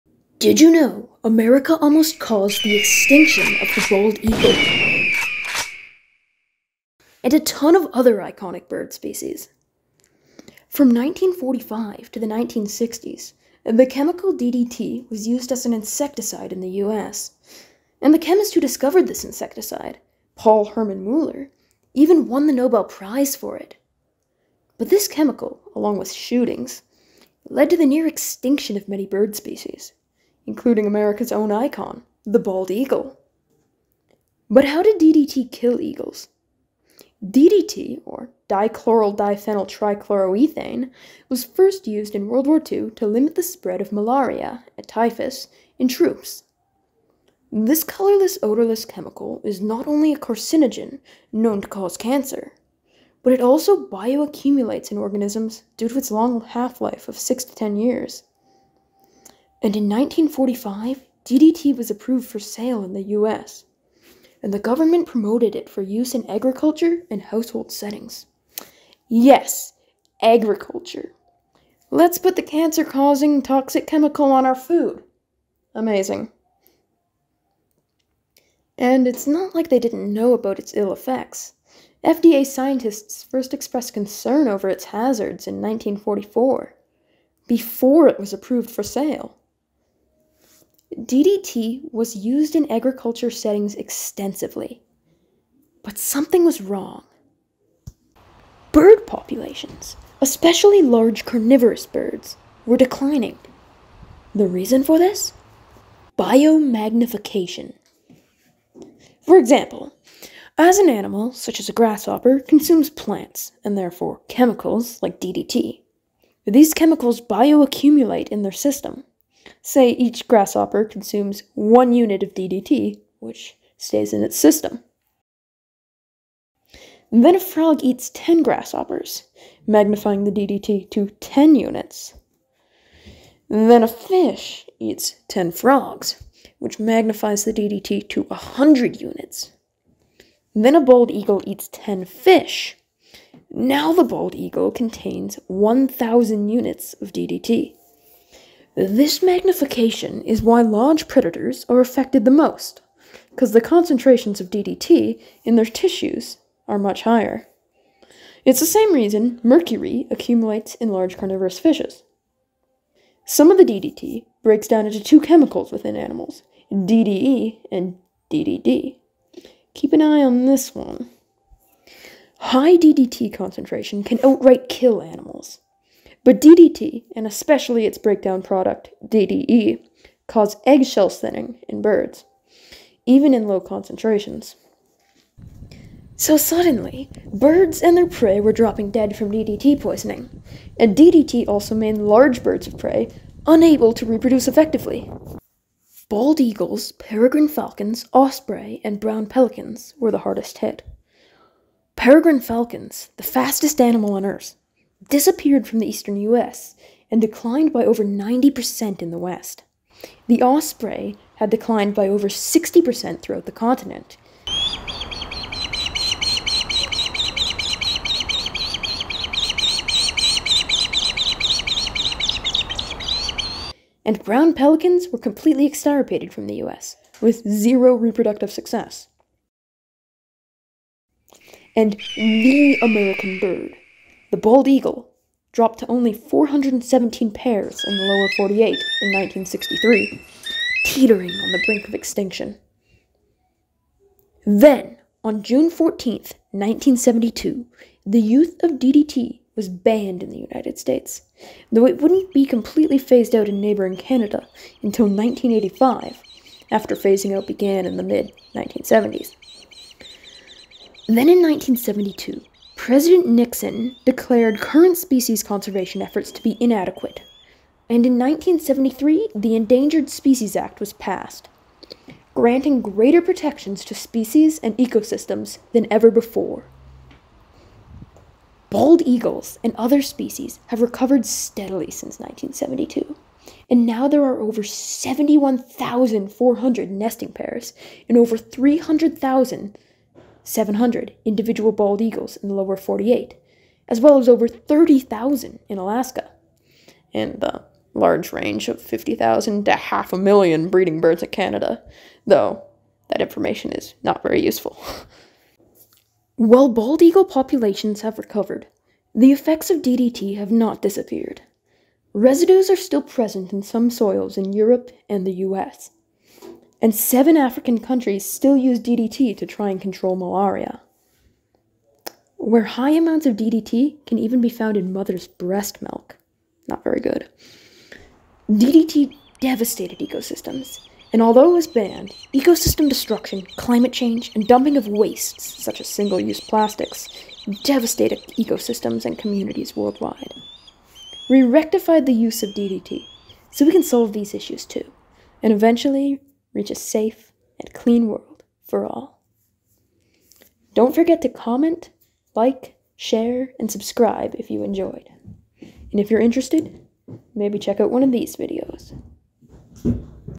How the Bald Eagle nearly became Extinct!
America almost drove the bald eagle to extinction during the 1900s. The main cause was the pesticide DDT and its environmental impact.

Bald eagles are a national symbol of the United States and have a ton of environmental protections today.

But in the early 1900s, the bald eagle was driven to near extinction. Back then, eagles were shot as they were viewed as pests that stole fish. Shootings combined with the environmental impact of DDT on bald eagles caused the near extinction of this species.

The decline of the peregrine falcon, the bald eagle, the brown pelican, the osprey, and the near-extinction of these interesting birds.

Birds such as the bald eagle were the most impacted by DDT.

Animal analysis explains the life history, phylogeny, and interesting facts about animals.

On the Brink of Extinction, explains the history of animals that went extinct, or almost went extinct in recent times, mainly due to human interference.
On this episode: The environmental impact of DDT and how America nearly drove the bald eagle to extinction.

00:00 Causes of Decline
01:48 Biomagnification
02:45 DDT impact
04:03 Ban of DDT and recovery